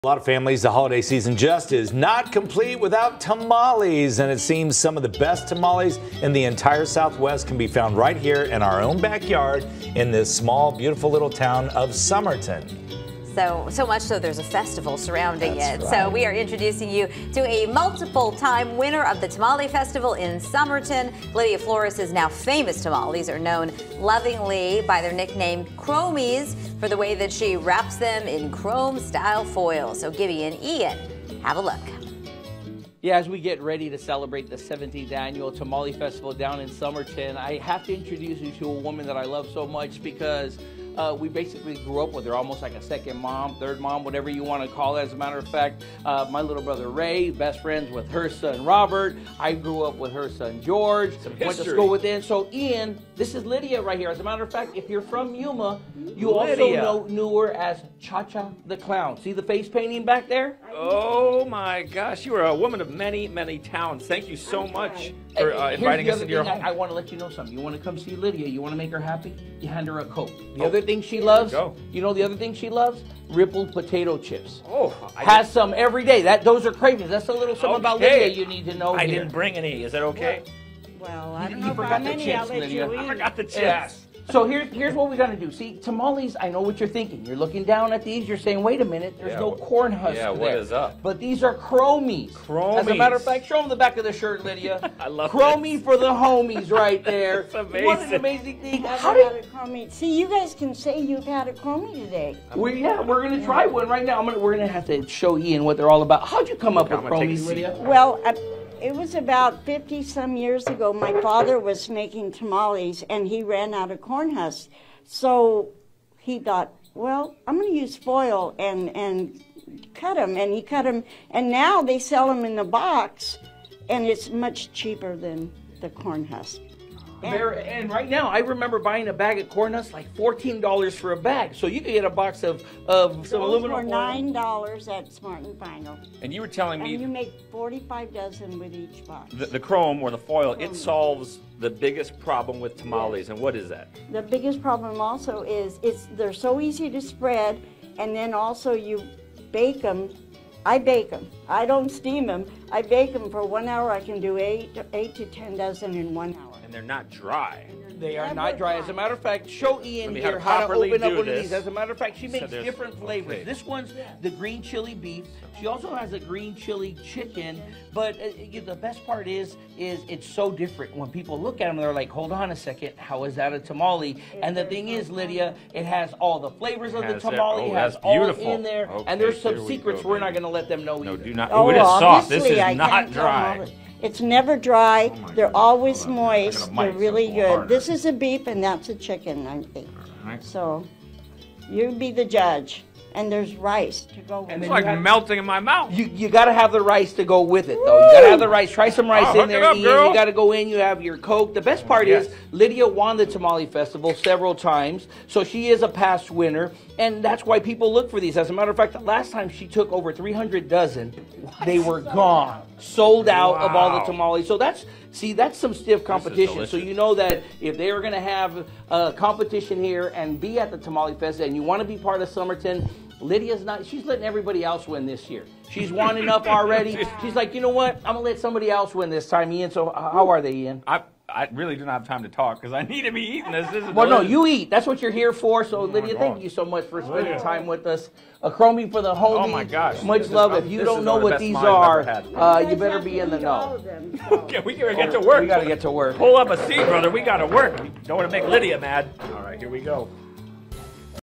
Meet the Yuma County woman who makes award-winning tamales
Meet Lydia Flores, a past winner of the Tamale Festival.